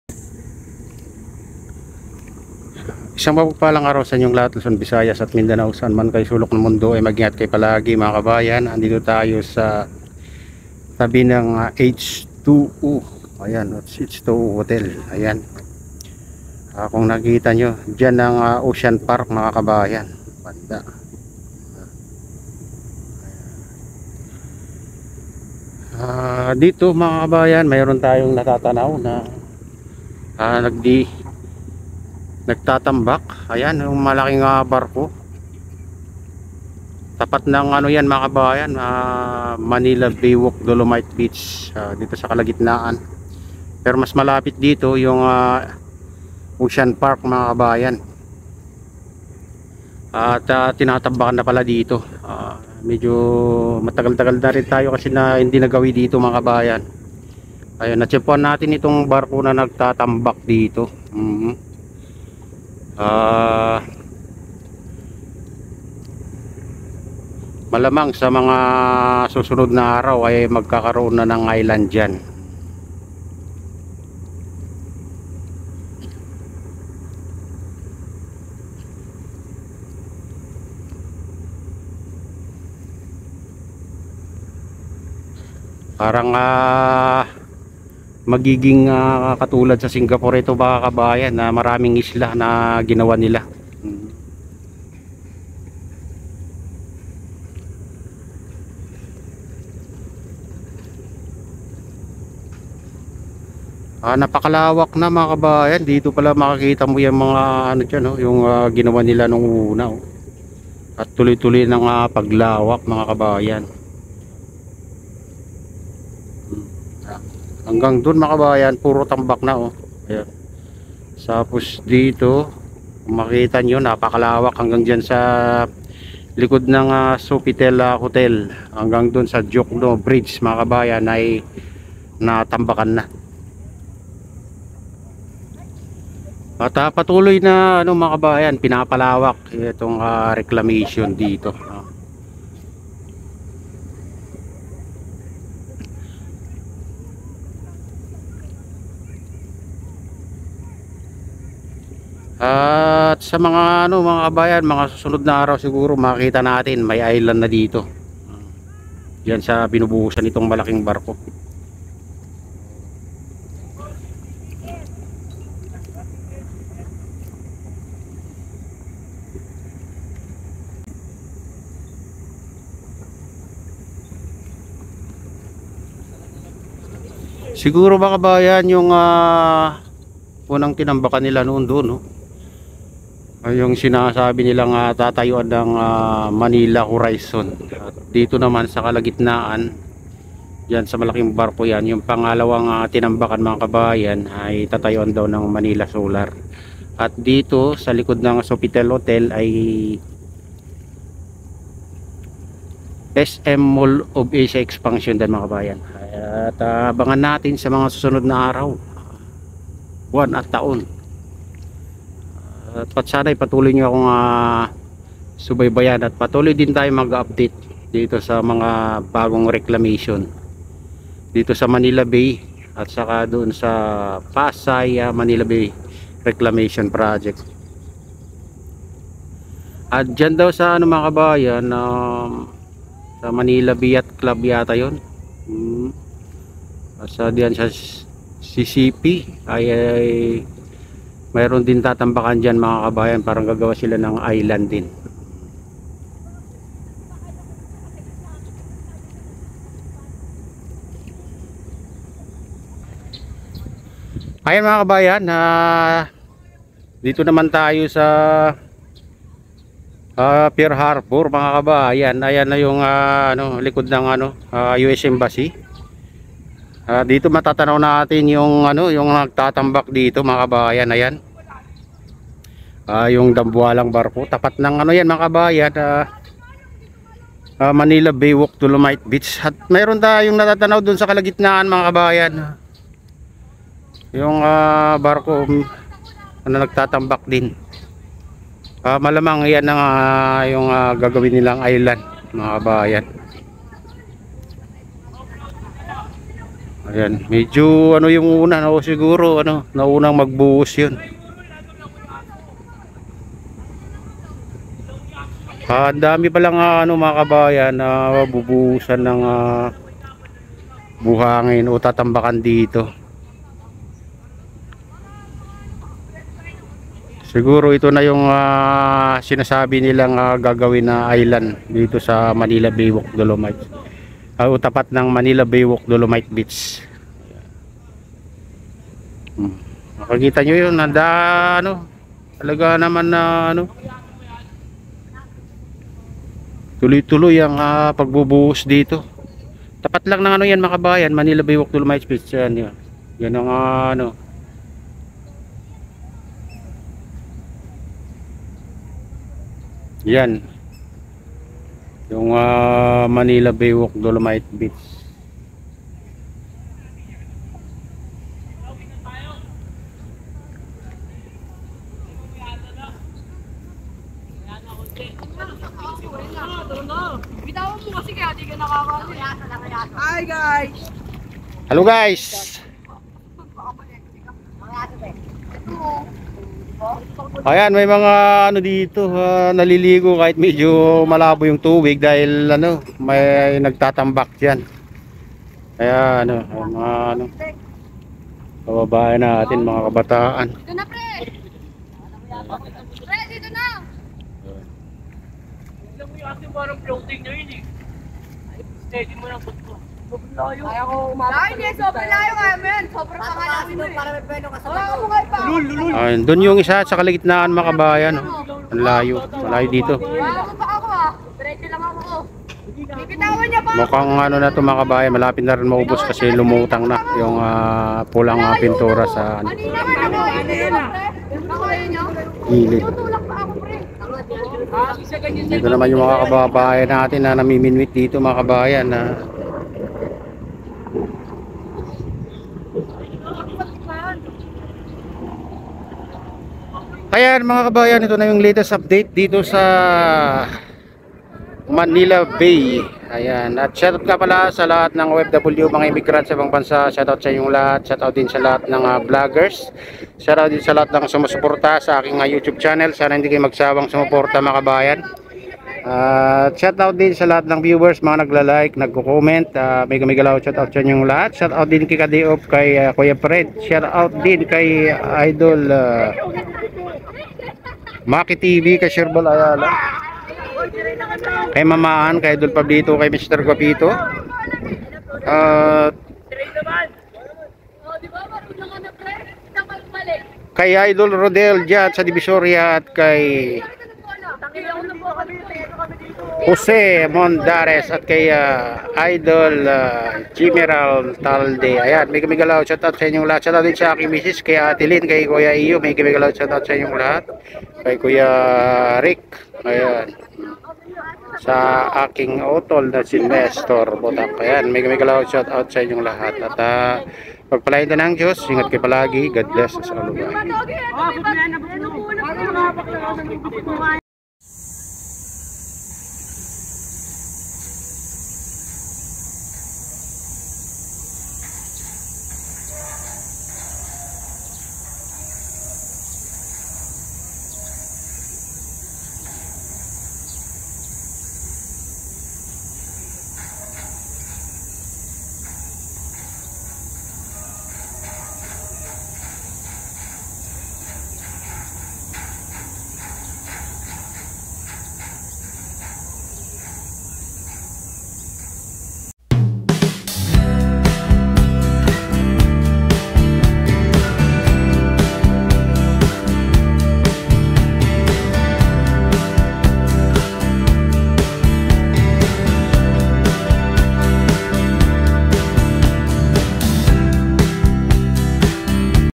tapat ng manila bay WALK DOLOMITE beach tuluyan ng tatambakan update july 6,2023
manilabayfrontreclamationprojectupdatejuly6,2023#@marcelobutacvlog